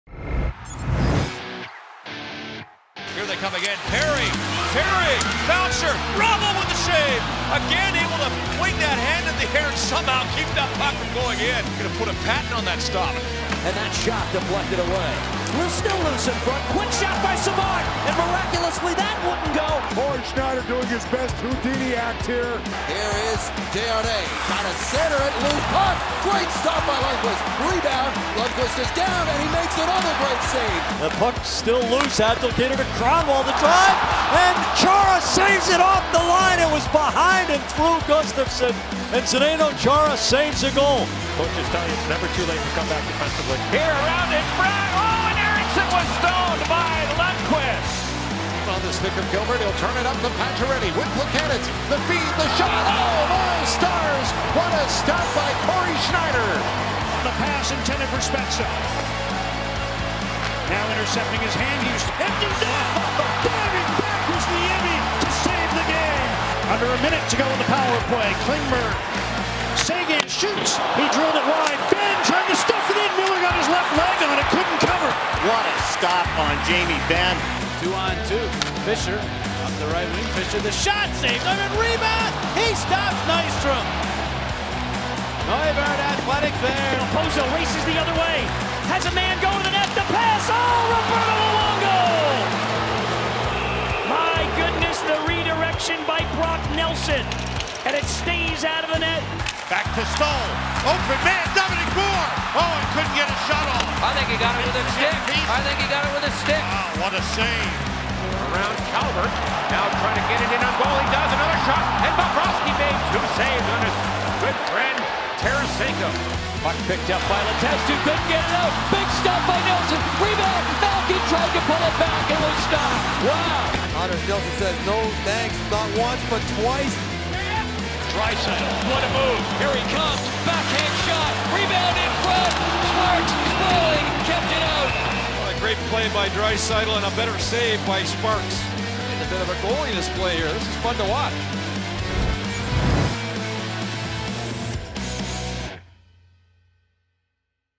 Saves of the Week: Schneider does his best Houdini act
Now you see the puck, now you don't when you take on the Devils star netminder, he and his fellow keepers make some miraculous moves in this edition of saves of the week.